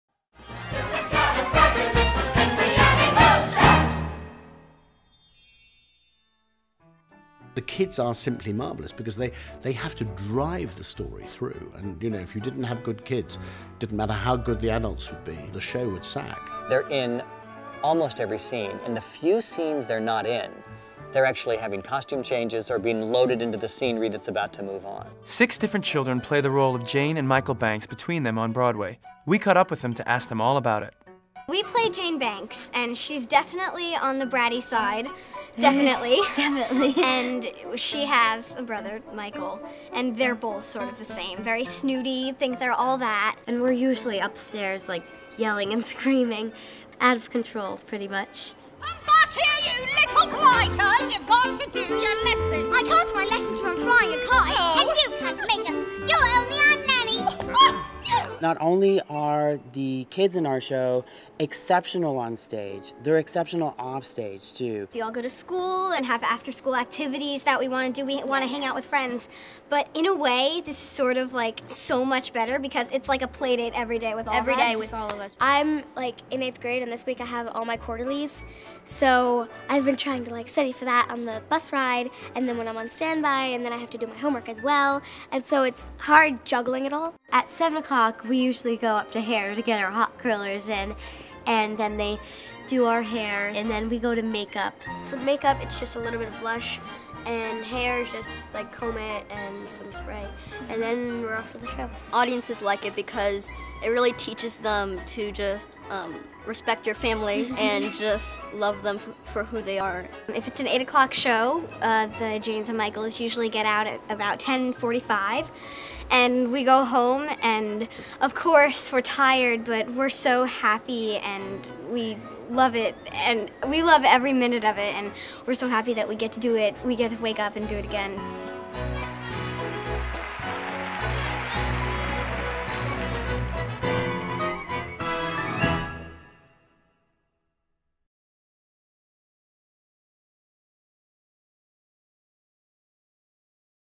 MARY POPPINS: Being Jane and Michael Banks
Did you know that there are six different children that play Jane and Michael Banks for the Broadway production of MARY POPPINS?  Watch this video and learn more about a day in the life of being Jane and Michael Banks.

To discover more about the hit Broadway musical MARY POPPINS, visit marypoppins.com